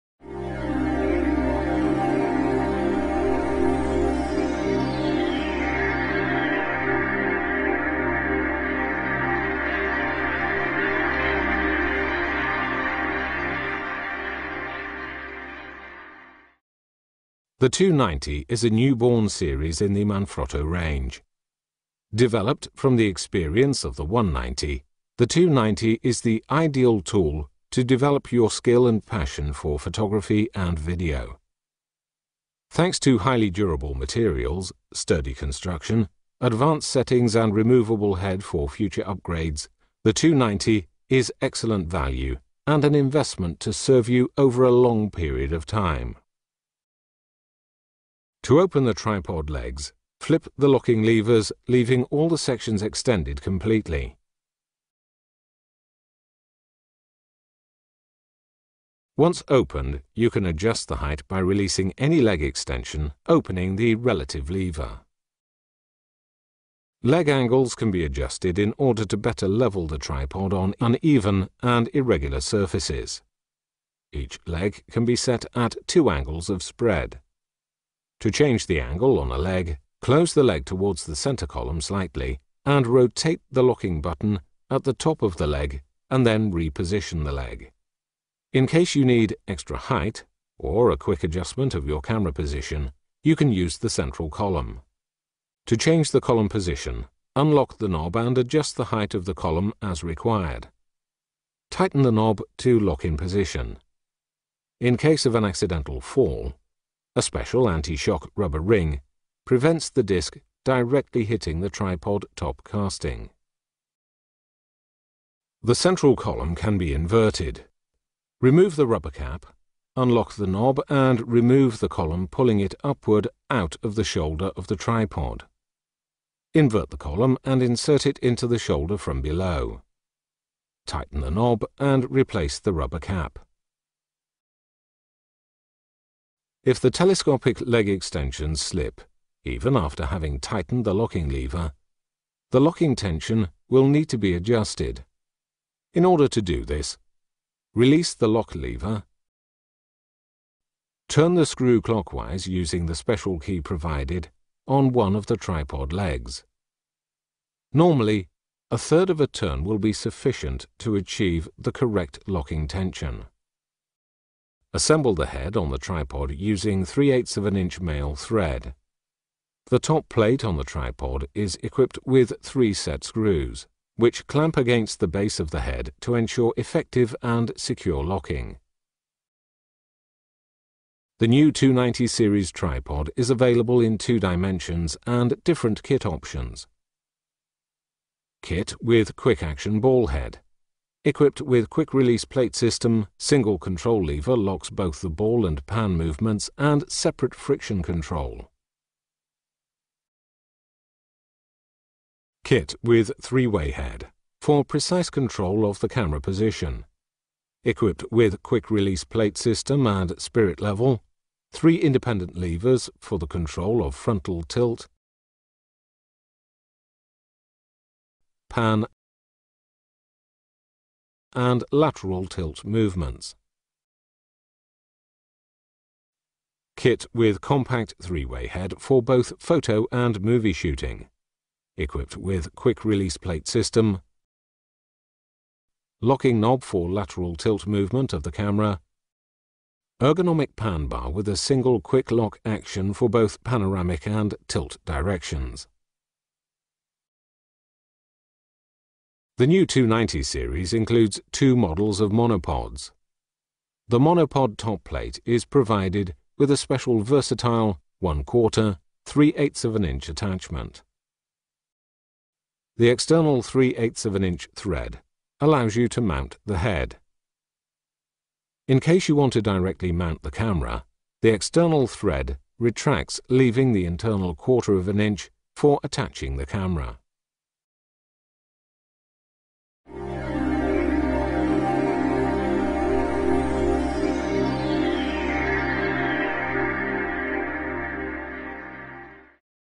Tripie Manfrotto 290 Dual (MT290DUA3)
Tripié Manfrotto para foto columna con sistema de 90 ° para tomas con mayor perspectiva 4 ángulos de posicionamiento. Nuevo agarre de goma Disco de metal con logo Manfrotto para asegurar su autenticidad, diseño italiano pensado para mejorar la funcionalidad.